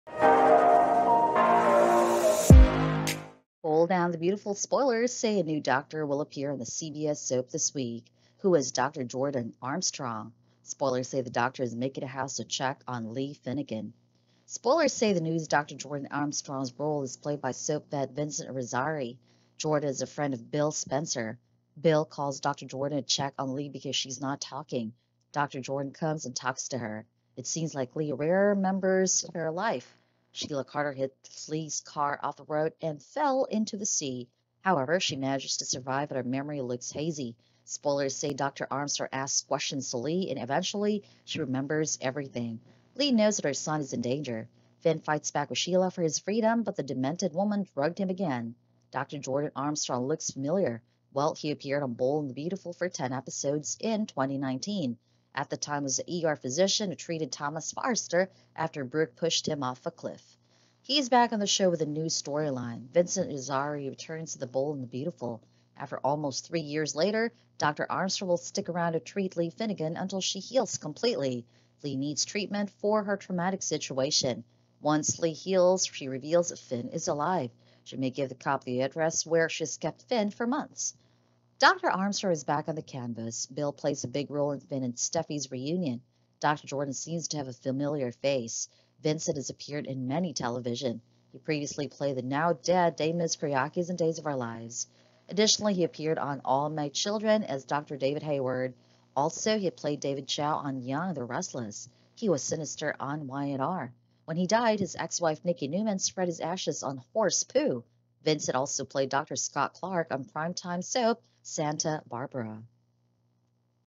Bold and the Beautiful Comings and Goings: Vincent Irizarry Returns as Dr. Jordan to Save Li
Bold and the Beautiful spoilers say a new doctor will appear on the CBS soap this week. Who is Dr. Jordan Armstrong? Bill Spencer calls a doctor to check on Li Finnegan. Jordan is a friend of Bill Spencer. How is Li Finnegan?